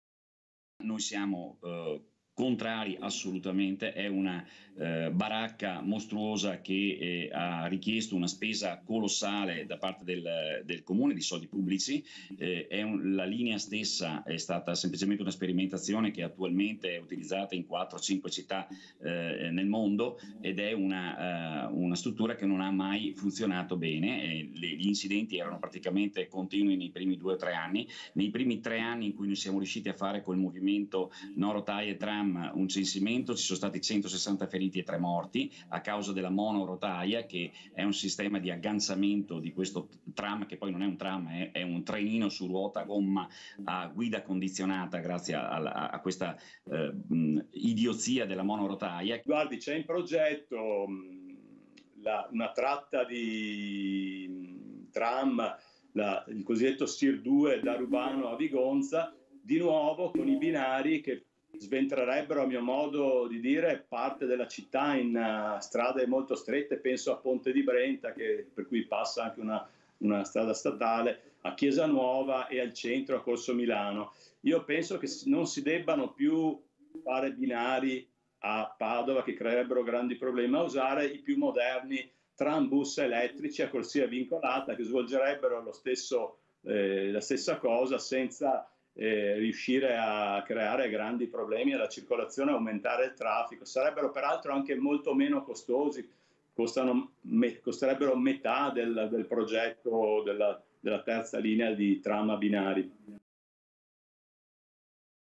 TRAM PADOVA: TEMA CALDO DELLA CAMPAGNA ELETTORALE | 28/04/2022
TG Padova Sera 28/04/2022 - PADOVA - La realizzazione della nuova linea del Tram si conferma tema caldo della campagna elettorale, anche oggi i candidati sindaco in gara per la poltrona di Palazzo Moroni, invitati a Tutto Mattino, hanno espresso chiaramente le loro posizioni sulla questione tanto dibattuta - Intervistati PAOLO GIROTTO (Candidato Sindaco Padova), FRANCESCO PEGHIN (Candidato Sindaco Padova) (Servizio di Fabio Mason) - Segui Telenordest anche sul digitale terrestre! Visita il sito